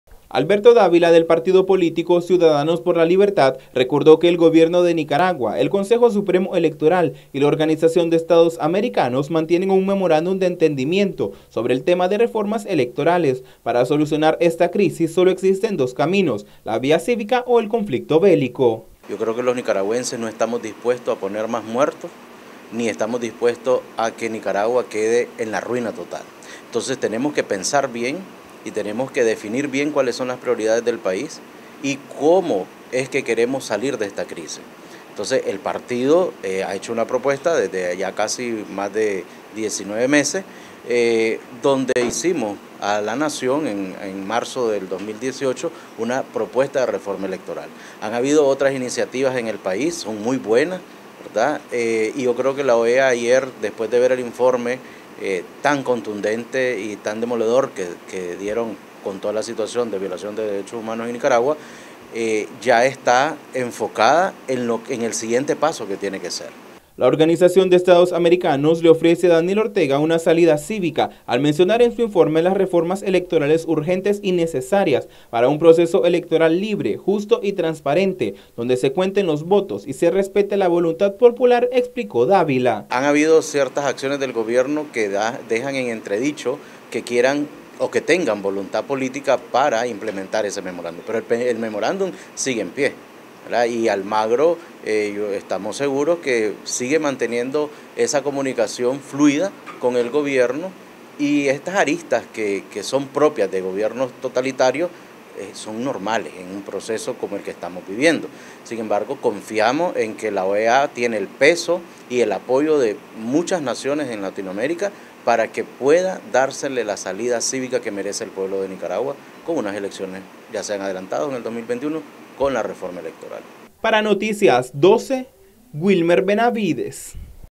OEA INSISTE EN REFORMAS ELECTORALES
Poco o nada se ha avanzado en el memorándum de entendimiento entre Nicaragua y la OEA sobre reformas electorales.